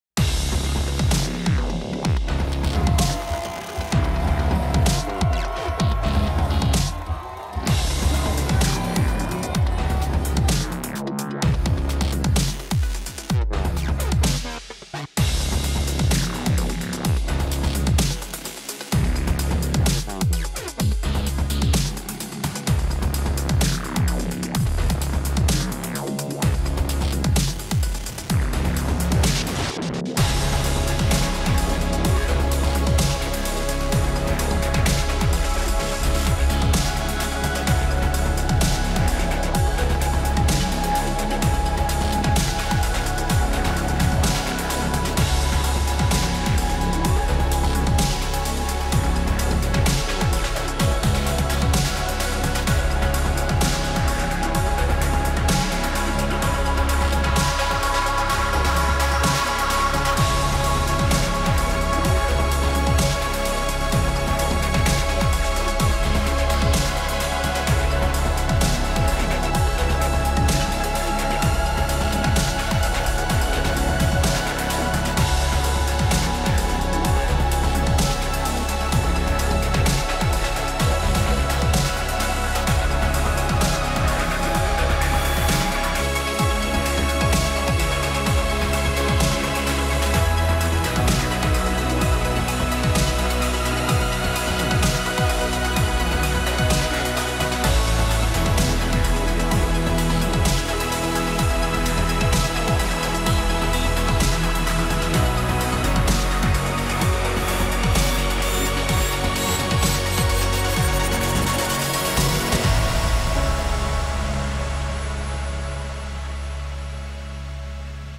Ljubljana v Saskatoon - Final Highlights - FIBA 3x3 World Tour Saskatoon Masters 2017
Relive the best moments of the FIBA 3x3 World Tour Saskatoon Masters final between 3x3 World Tour reigning champs Ljubljana (SLO) and hosts and local favorites Saskatoon (CAN)!